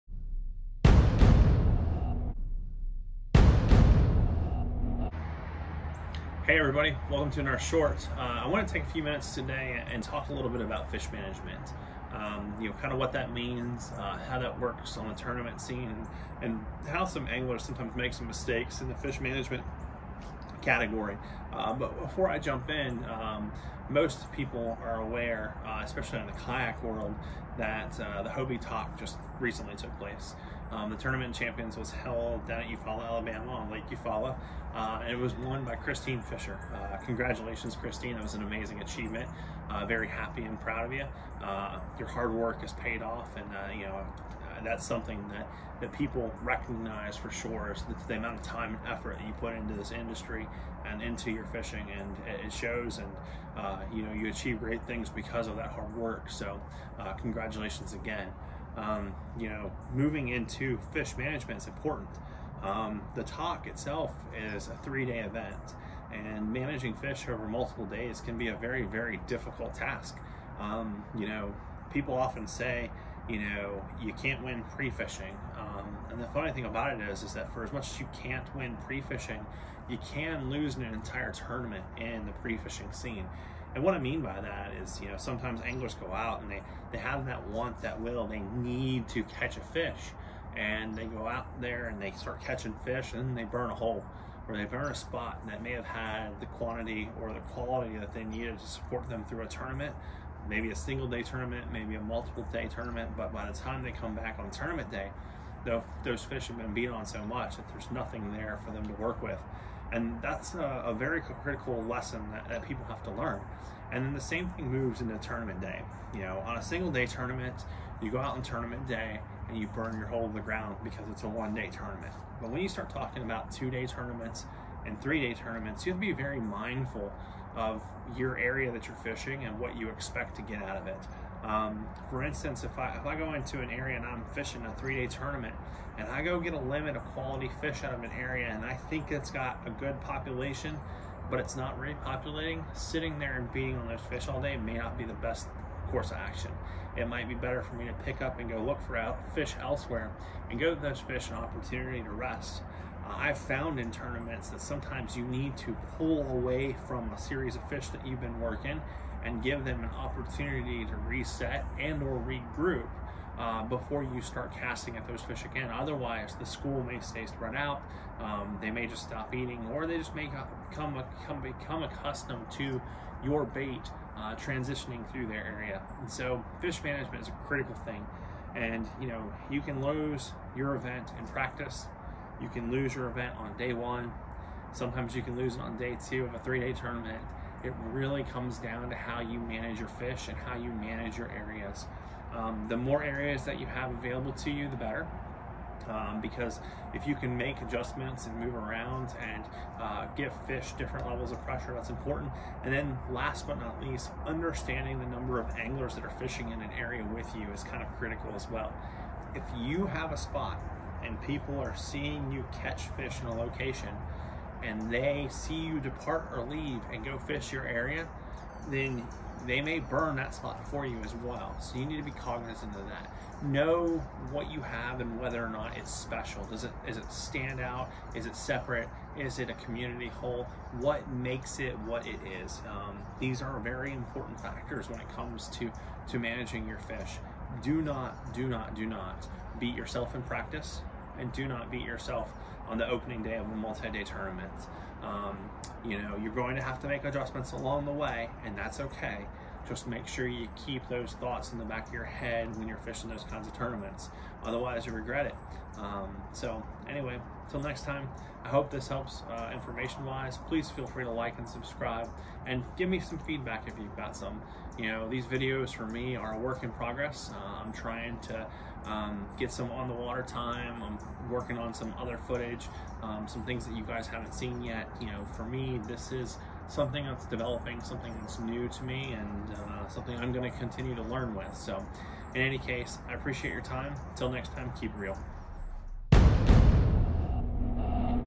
S1 E5 - Importance of Fish Management
Short on fish management. Nothing super spectacular here, but something that is very important. So what people need to take away is to manage their fish effectively, don’t burn your spots in practice and be cognizant of those around you. Please feel free to throw your comments below on what special things you do for fish management. Tight lines everyone!